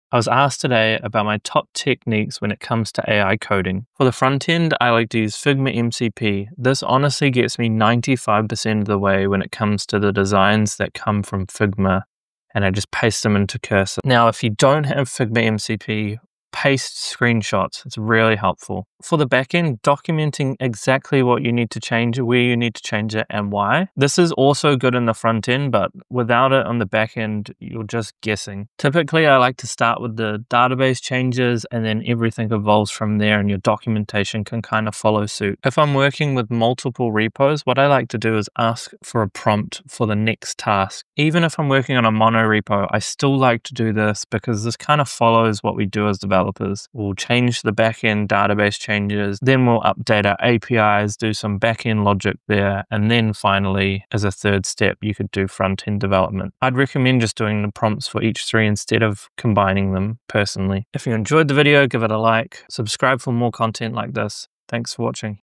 My Top AI Coding Techniques for Frontend & Backend
This is how I get the most out of cursor